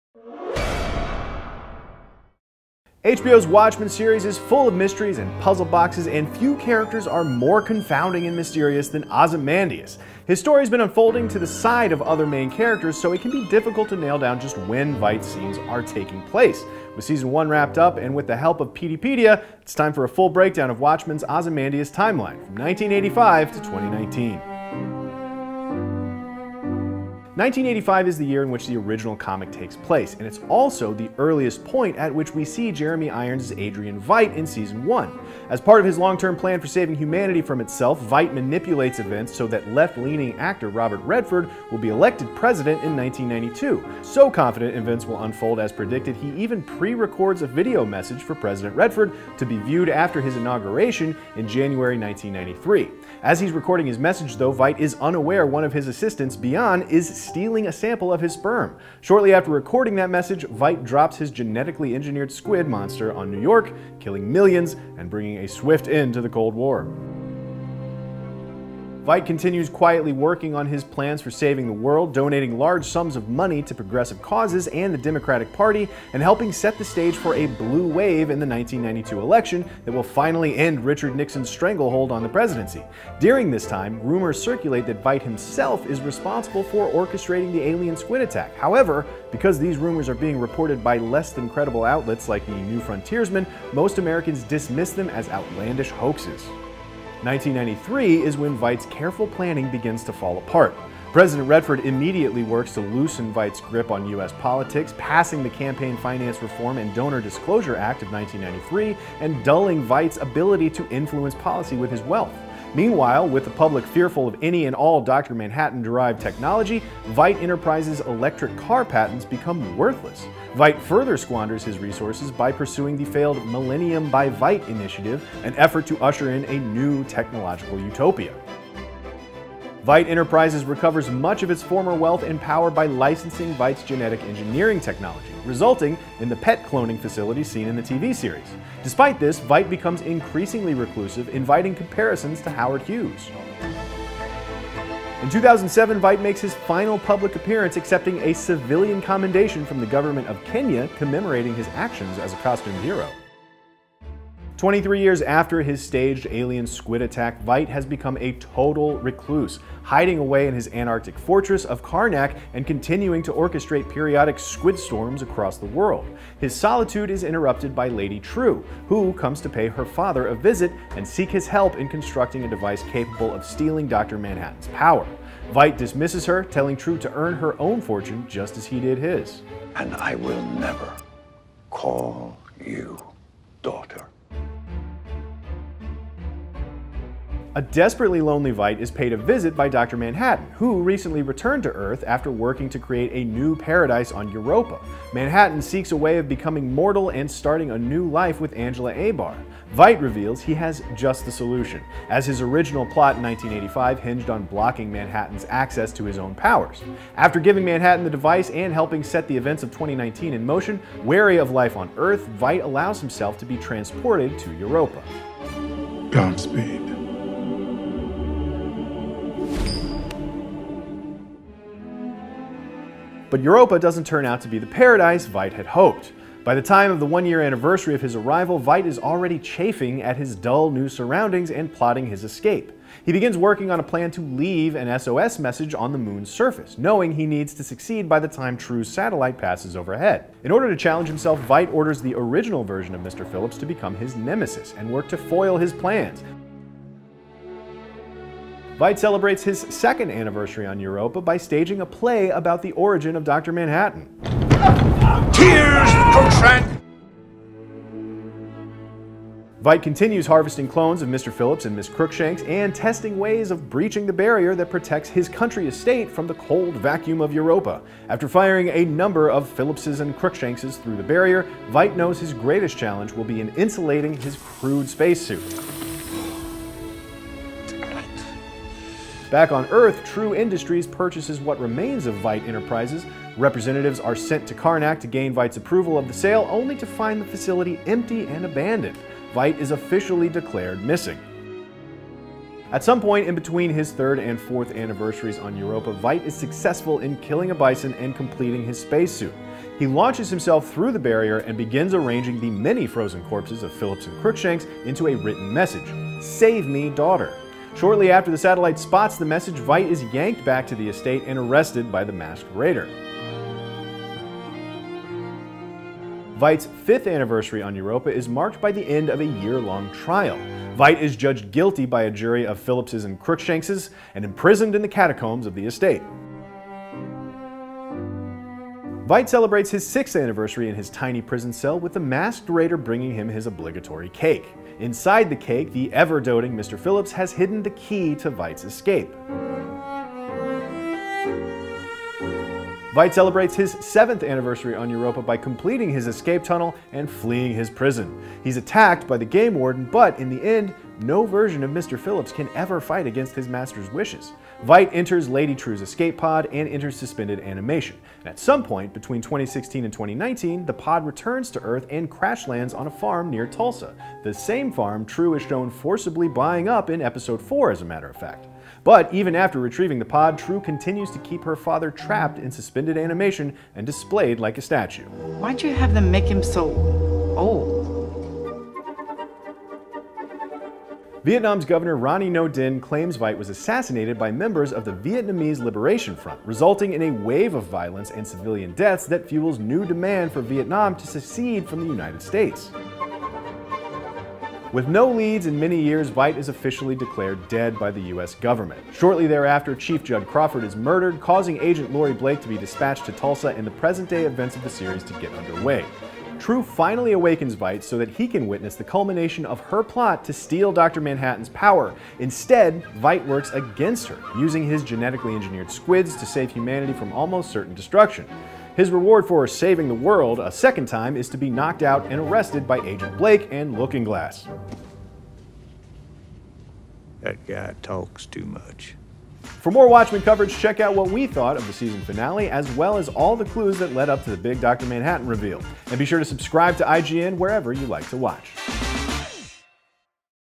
Watchmen: Ozymandias' Confusing Timeline Explained
Confused as to how Ozymandias fits into Watchmen: Season 1? We break down Adrian Veidt's timeline from 1985 to 2019, from Karnak to Jupiter's moon Europa and back to Earth again.